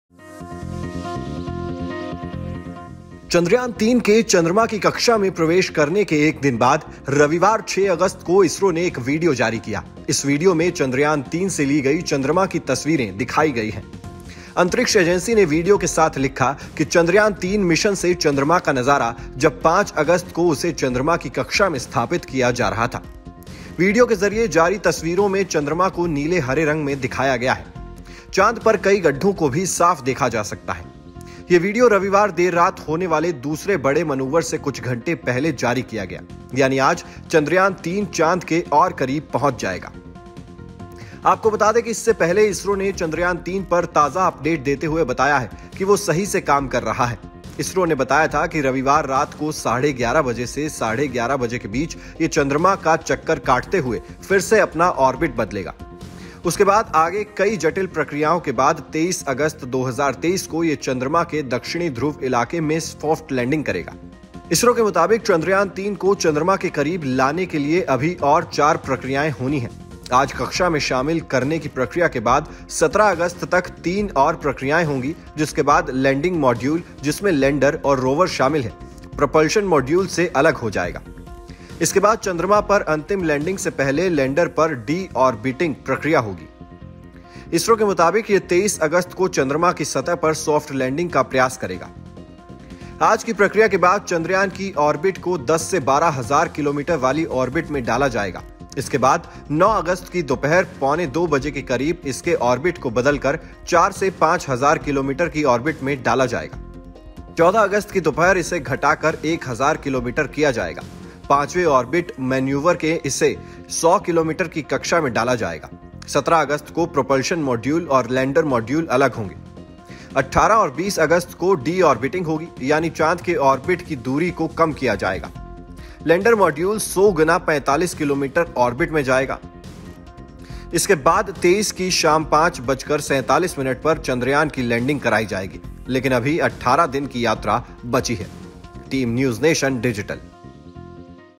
Chandrayaan3 First Images of Moon: Chandrayaan 3 ने भेजी चांद की तस्वीरें ISRO ने शेयर किया वीडियो
चंद्रयान-3 के चंद्रमा की कक्षा में प्रवेश करने के एक दिन बाद रविवार 6 अगस्त को इसरो ने एक वीडियो जारी किया. इस वीडियो में 'चंद्रयान-3' से ली गईं चंद्रमा की तस्वीरें दिखाई गईं हैं. अंतरिक्ष एजेंसी ने वीडियो के साथ लिखा कि 'चंद्रयान -3' मिशन से चंद्रमा का नजारा, जब 5 अगस्त को उसे चंद्रमा की कक्षा में स्थापित किया जा रहा था. वीडियो के जरिए जारी तस्वीरों में चंद्रमा को नीले हरे रंग में दिखाया गया है. चांद पर कई गड्ढों को भी साफ देखा जा सकता है. ये वीडियो रविवार देर रात होने वाले दूसरे बड़े मनूवर से कुछ घंटे पहले जारी किया गया. यानी आज चंद्रयान-3 चांद के और करीब पहुंचाया जाएगा.

ISRO released a video on Sunday, August 6, a day after Chandrayaan-3 entered the Moon's orbit. In this video pictures of Moon taken from 'Chandrayaan-3' are shown. The space agency wrote with the video that the view of the Moon from the 'Chandrayaan-3' mission, when it was being installed in the Moon's orbit on 5 August.In the pictures released through the video, the moon is shown in blue-green color. Many pits can also be seen clearly on the moon. This video was released a few hours before the second big maneuver to be held late on Sunday night. That is, today Chandrayaan-3 will be brought closer to the moon.

News State Apps डाउनलोड करें:
Download Android and iOS Apps for more updates: